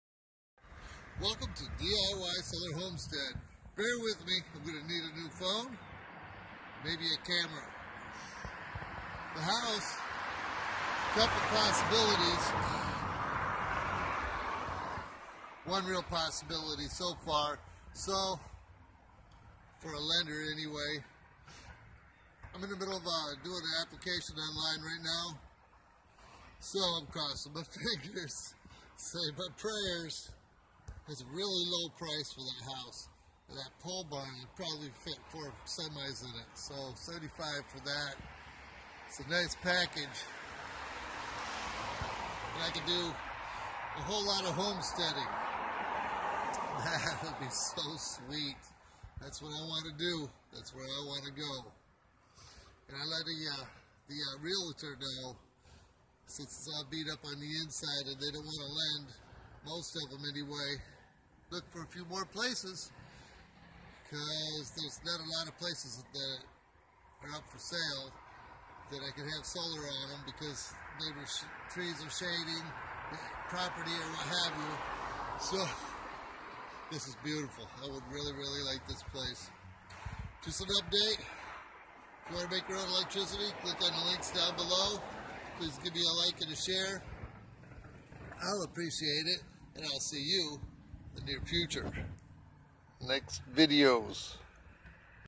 Filed an Application for the Homestead
Filed an Application for the Homestead. I would love this place. after I sell my house and cash in from the stock market before it crashes I just might owe 40 grand on it. That should keep the payment around 300 per month and that's what I pay now. I will just have 5 acres for animals and a food forest.